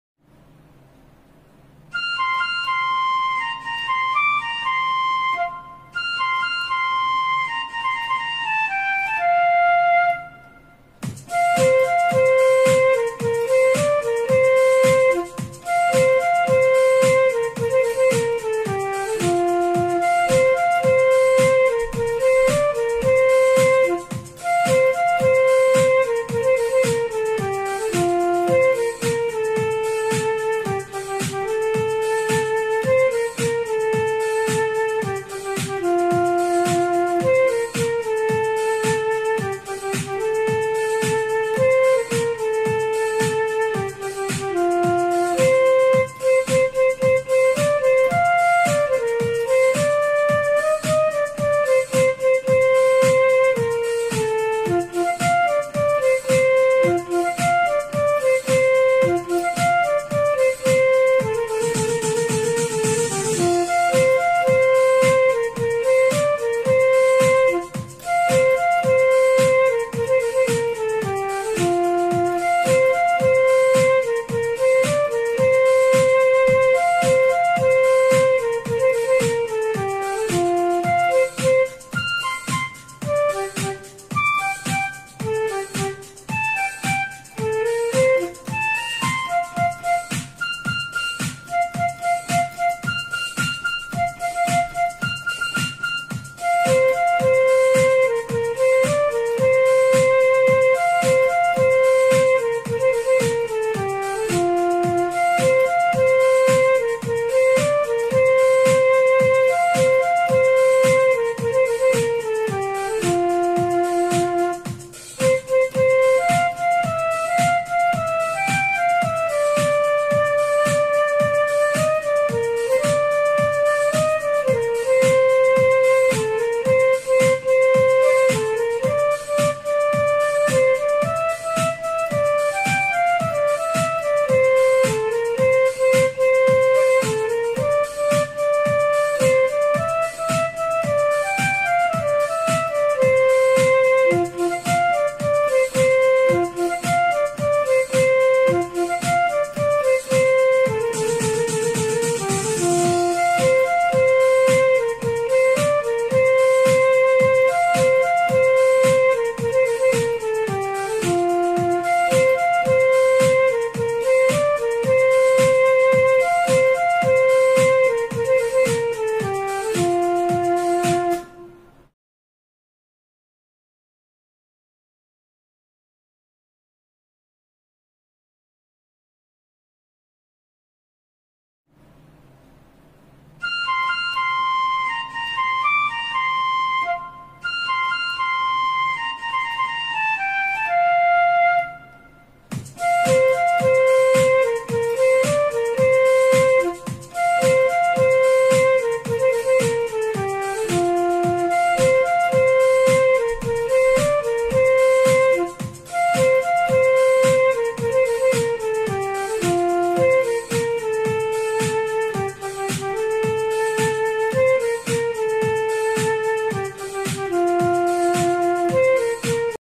A field trip for Nursery Kids
Bringing smiles wherever we go on our school field trip!